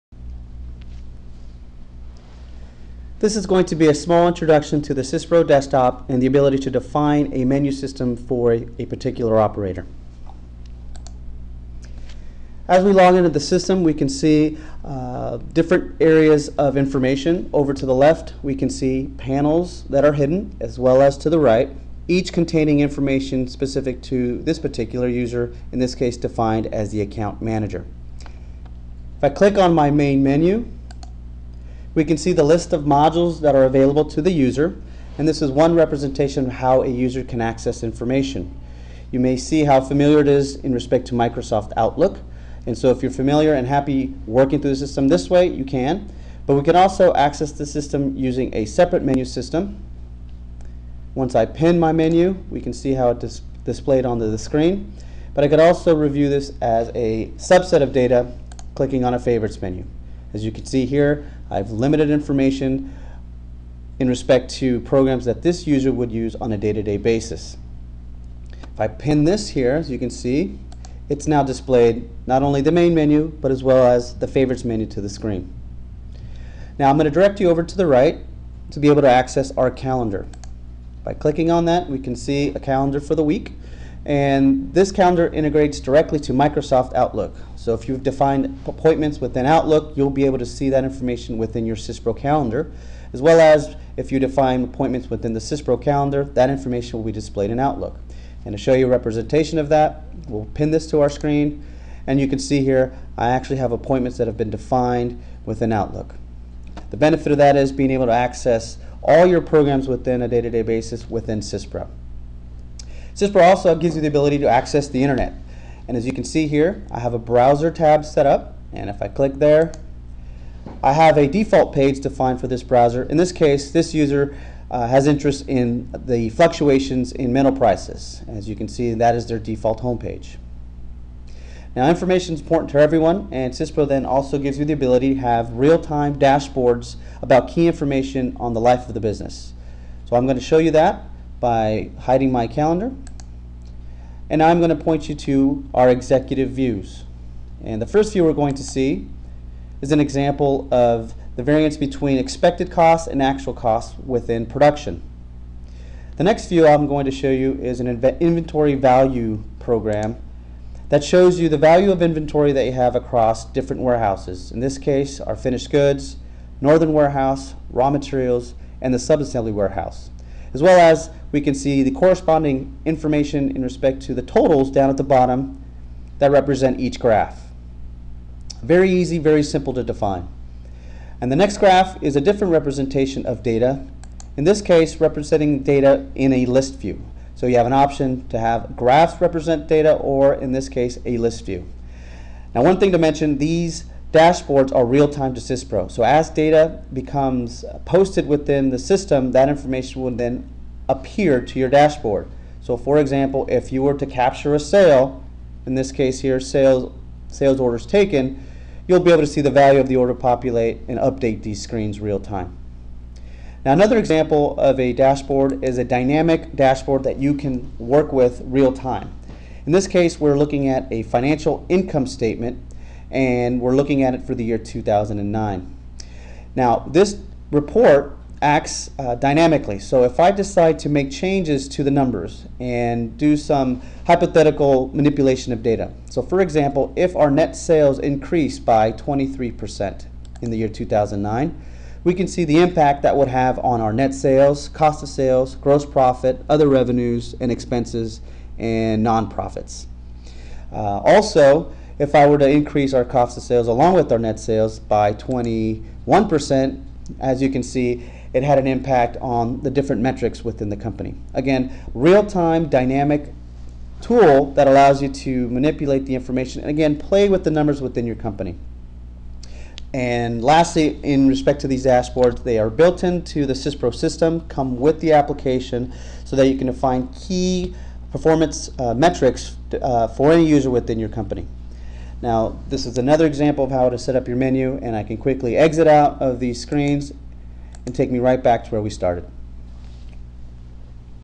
SYSPRO ERP Overview
Systems Advisory Services presents an overview of the look and feel of SYSPRO software